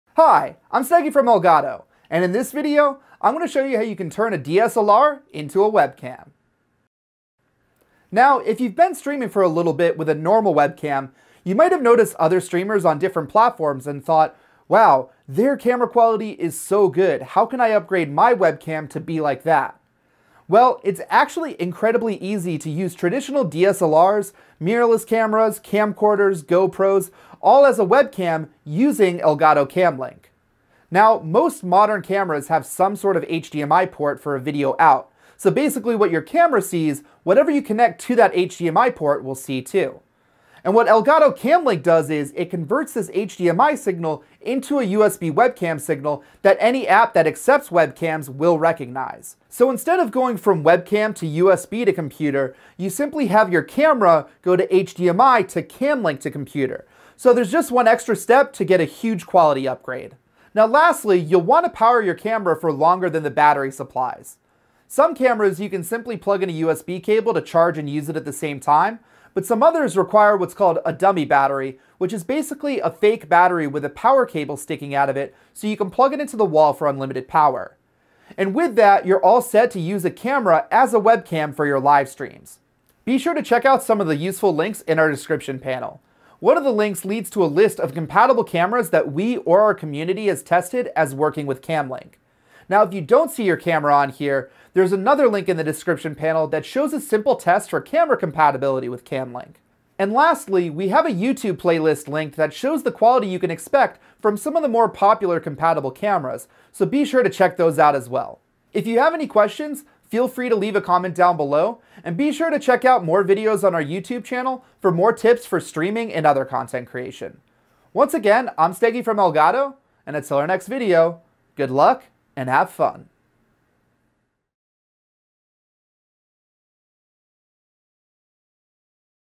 How to Use a DSLR as a Webcam
In this video we show how you can easily turn your DSLR, mirrorless camera, camcorder, or action camera into a Webcam using Elgato Cam Link.